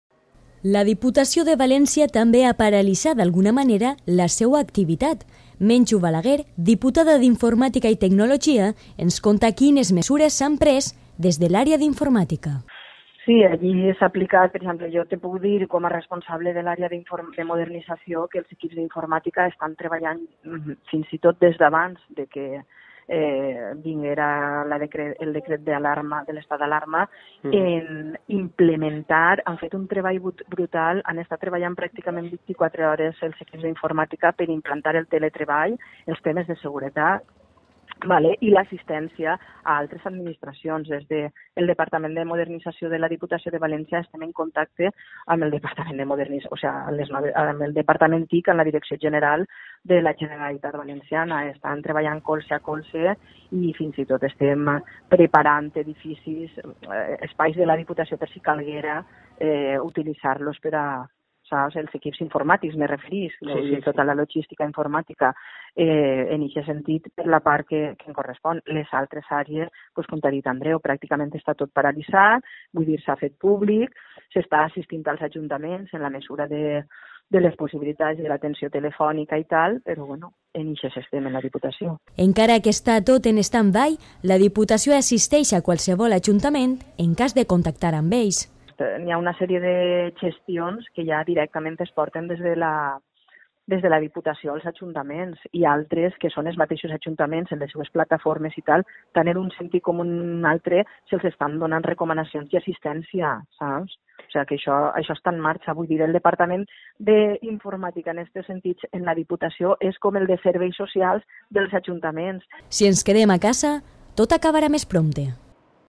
Diputació de València segueix treballant i donant suport als ajuntaments davant la crisi
Atén a qualsevol ajuntament en cas de posar-se en contacte amb ells.